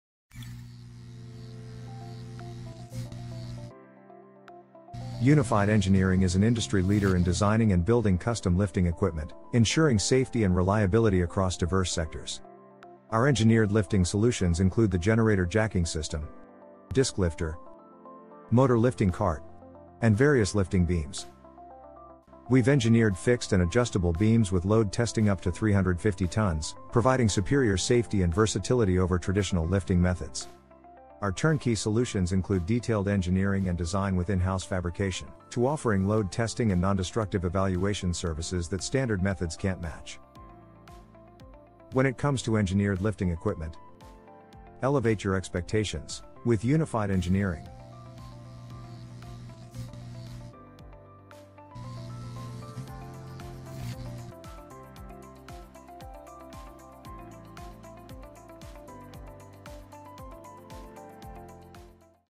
Lifting | Unified Engineering Products
As an industry leader in designing and manufacturing custom lifting equipment, Unified Engineering ensures safety and reliability across various sectors.

Some of our offerings include:
• Generator Jacking System
• Disc Lifter
• Motor Lifting Cart
• Various Lifting Beams

Our Turnkey solutions include:
• Engineering & Design
• In-House Fabrication
• Load Testing (Up To 350T)
• Non-Destructive Examination